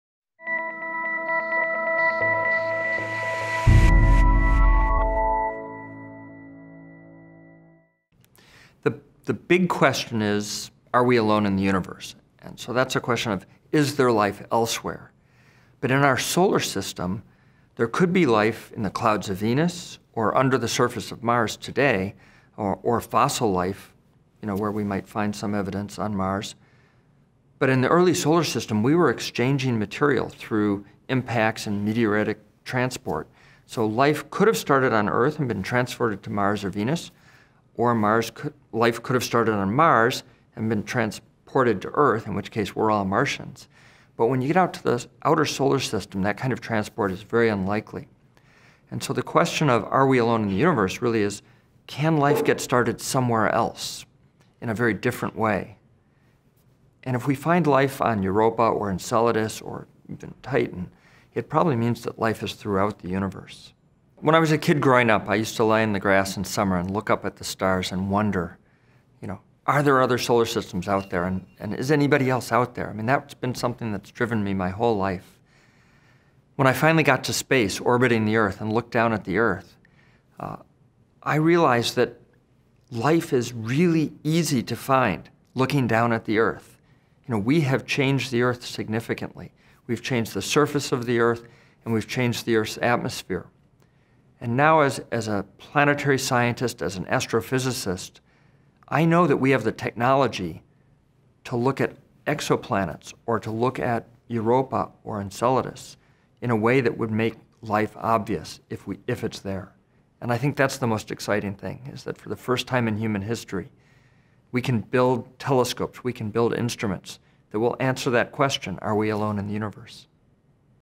John Grunsfeld: Does Alien Life Exist in the Universe?
In this Breakthrough Discuss interview, former astronaut and NASA administrator John Grunsfeld talks about the possibility of life in the Universe.